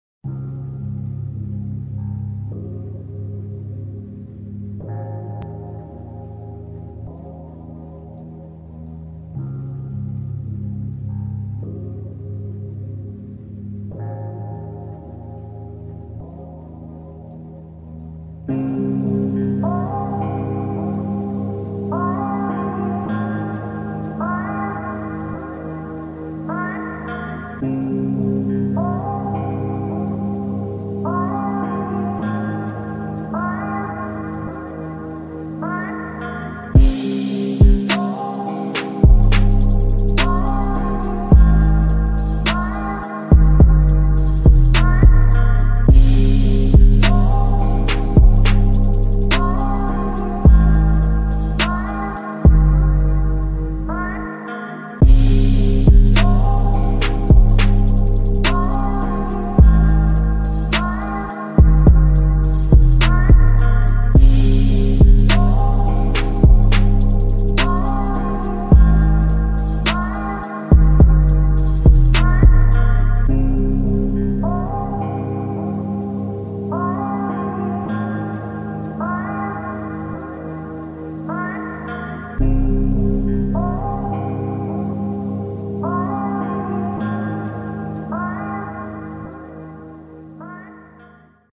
SMOKE | Sony a6500 / Sigma 16mm f1.4
Test video with new Sigma 16mm f1.4 DC NC. All clips are shot by hand-held. Shot with Sony a6500.